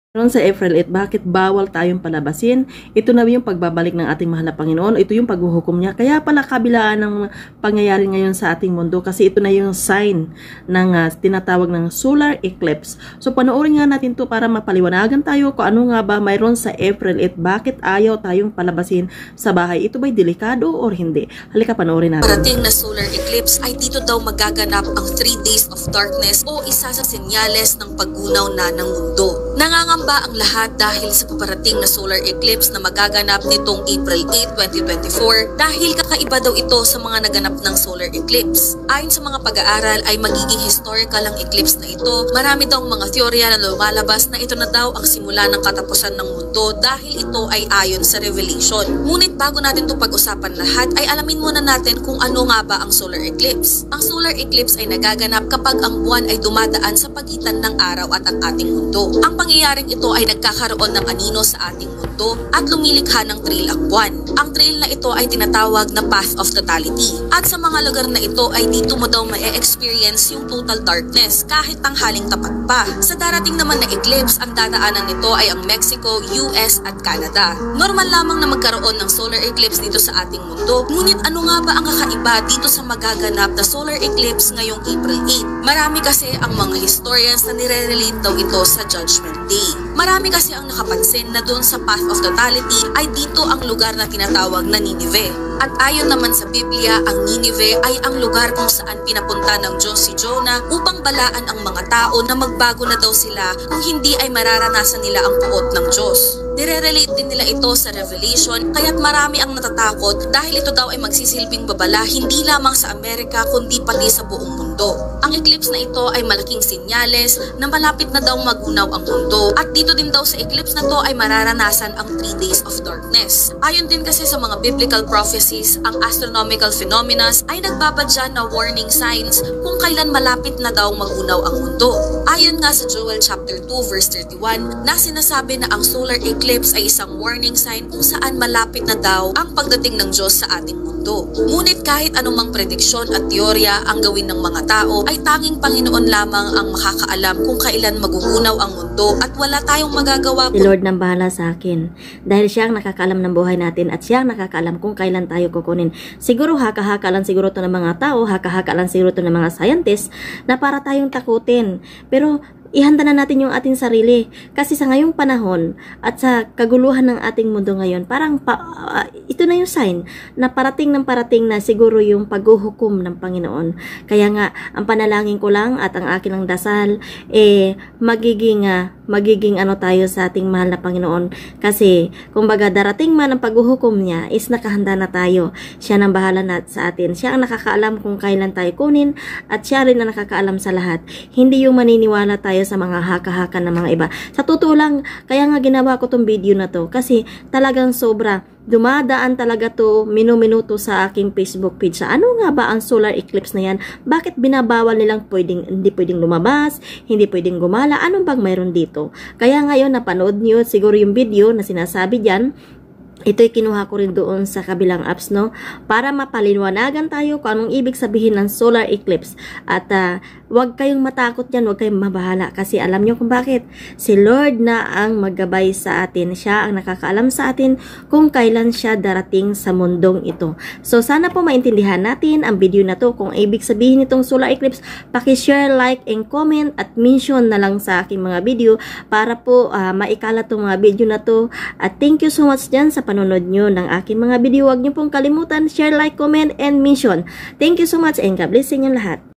April 8 bakit lumabas ang manga tao dahil sa solar eclipse
April 8 solar eclipse  bakit ayaw palabasan ang manga tao